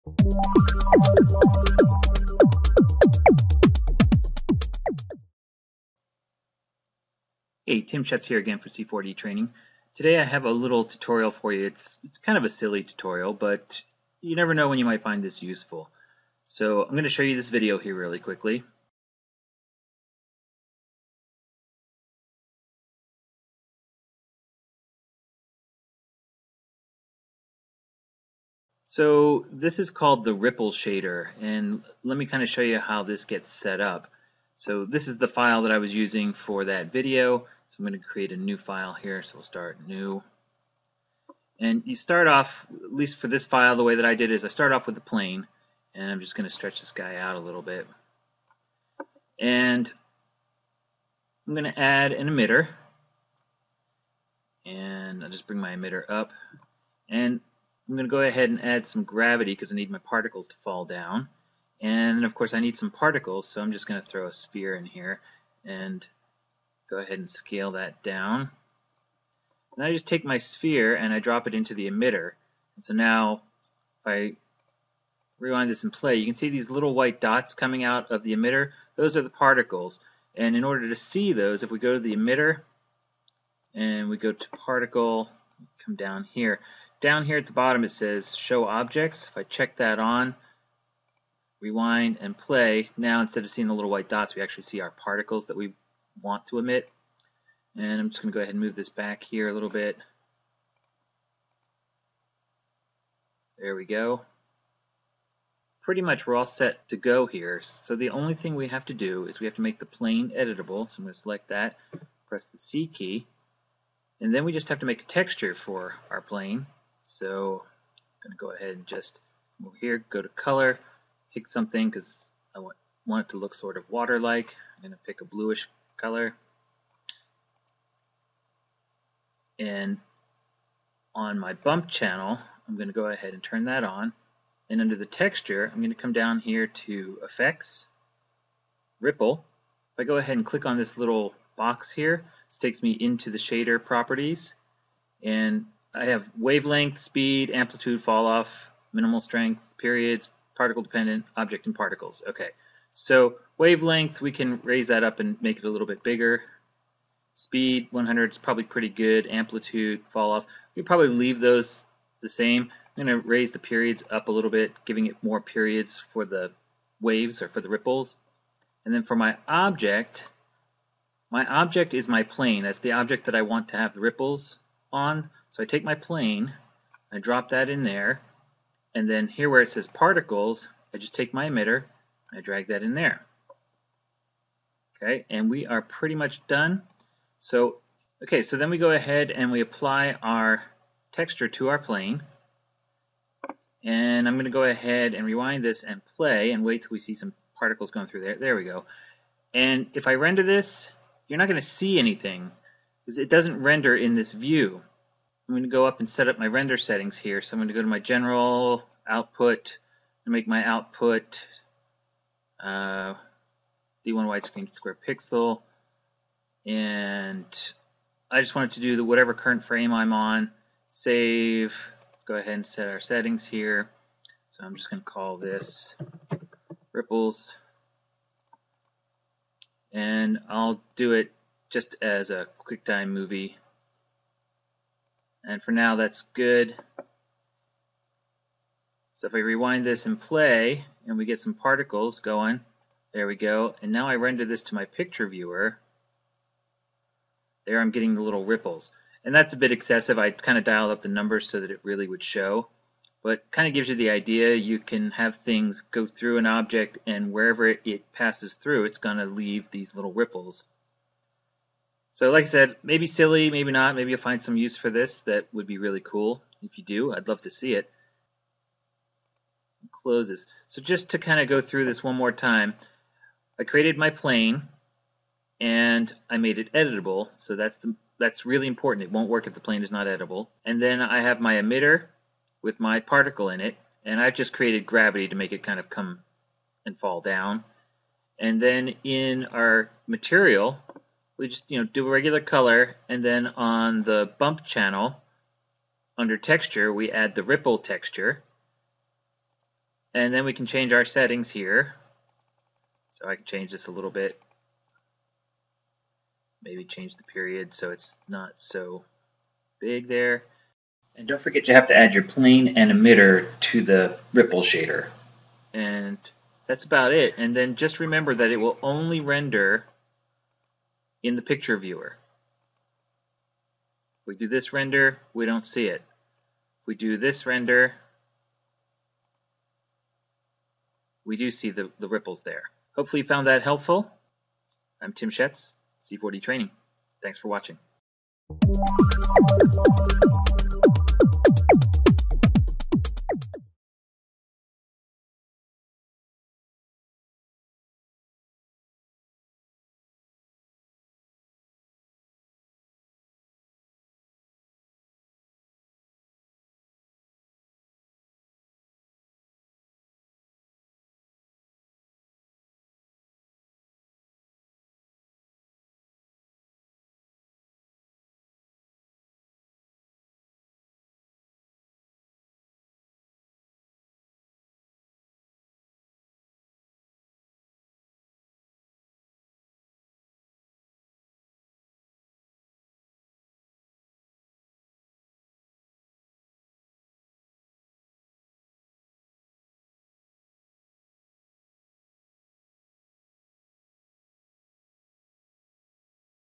Using the Ripple Shader in Cinema 4D
In this video tutorial, you'll learn how to use the Ripple Shader in Cinema 4D.Great for creating waves!  You can also put the shader in the displacement channel to get more of the effect.